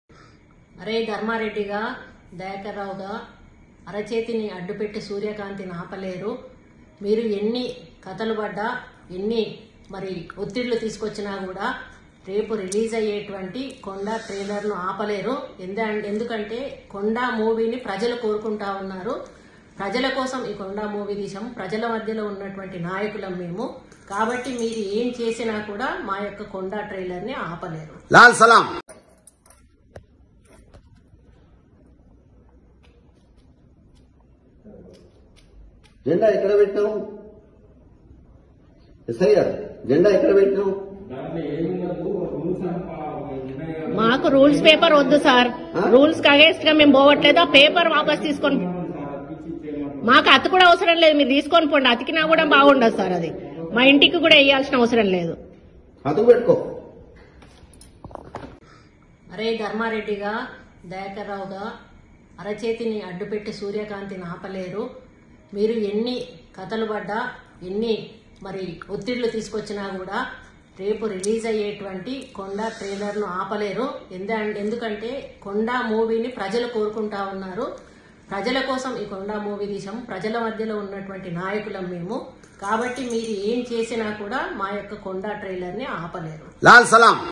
Konda Surekha STRONG Warning To Challa Dharma Reddy And Errabelli Dayakar Rao Over Kondaa Trailer
KondaSurekha STRONG Warning To Challa Dharma Reddy And Errabelli Dayakar Rao Over KondaaTrailer | RGV | Filmylooks